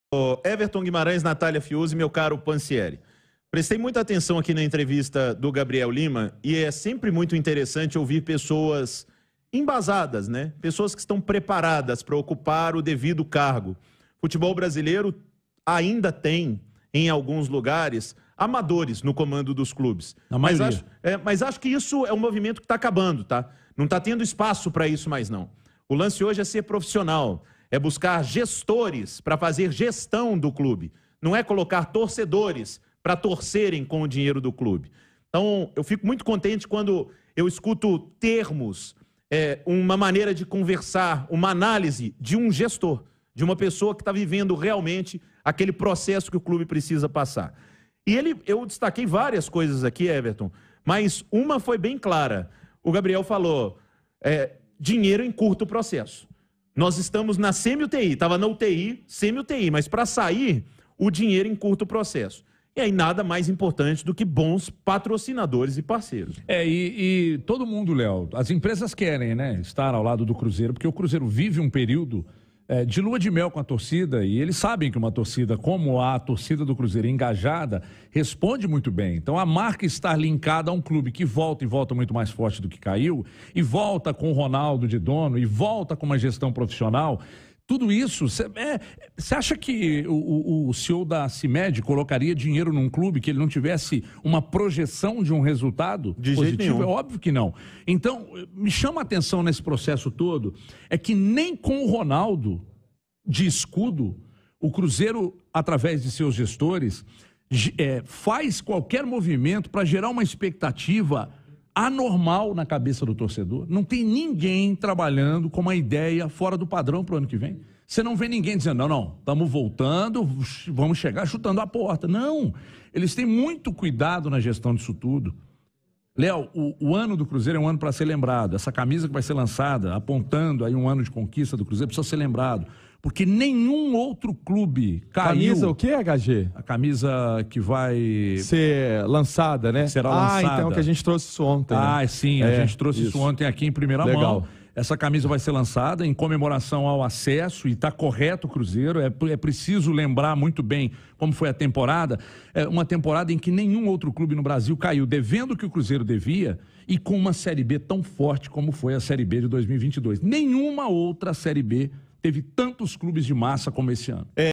QUAL O IMPACTO DA GESTÃO SAF DO CRUZEIRO PARA ARRECADAR INVESTIMENTOS AO CLUBE? LÉO E HG RESPONDEM
Os comentaristas Léo Figueiredo e Heverton Guimarães comentaram sobre a gestão do CEO Gabriel Lima e o novo acordo realizado com a CIMED.